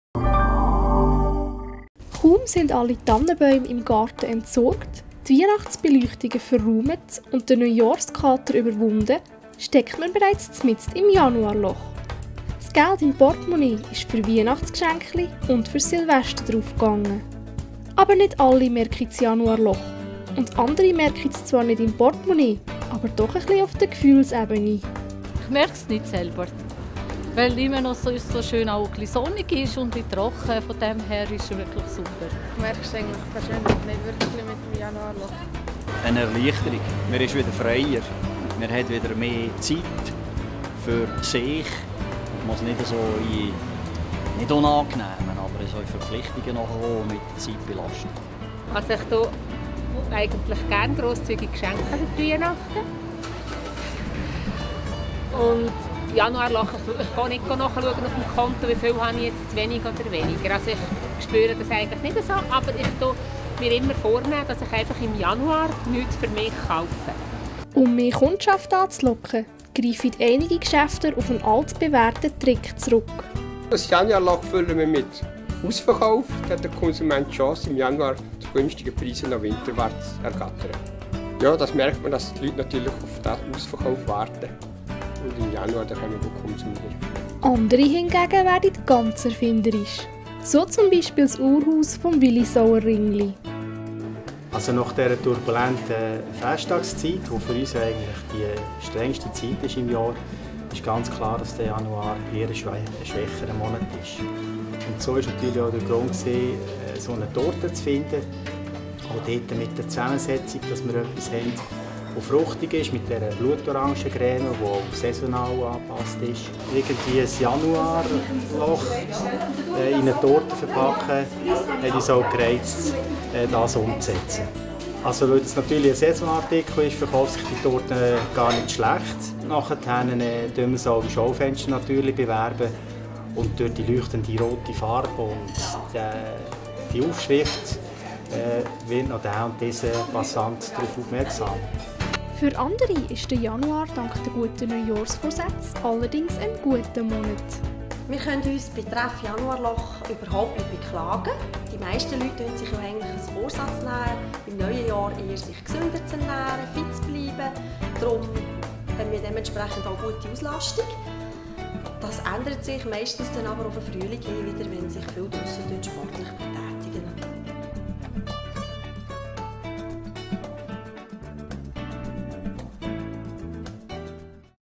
Januarloch
Kaum sind die Tannenbäume im Garten entsorgt, die Weihnachtsbeleuchtungen gelöscht und der Neujahrskater überwunden, steckt man auch schon mitten im Januarloch.
Doch wie stopft man es? Und "Wär hät's erfunde?" (natürlich die Schweizer, aber nicht Ricola)